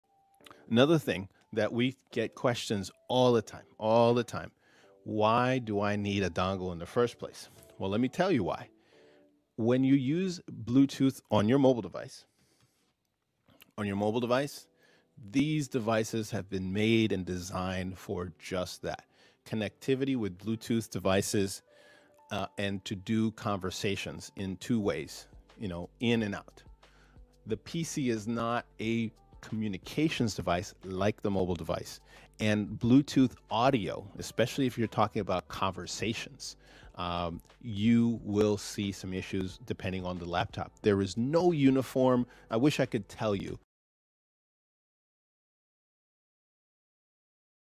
Reason why your Bluetooth 🎧 device is acting 🥴 fishy
If you're a gadget lover, chances are you've heard of Bluetooth dongles.

It's no secret that Bluetooth is one of the most popular ways to connect devices wirelessly.

Whether you're using a Bluetooth headset for hands-free calling or a Bluetooth speaker to play your favorite songs, this technology has become an essential part of our lives.

But what if you don't have a Bluetooth receiver built into your computer or device?

But if you're new to the world of technology, you may be wondering what a Bluetooth dongle is and why you always need one.

Many people don't know that using a Bluetooth dongle is always necessary to get the best results from their devices.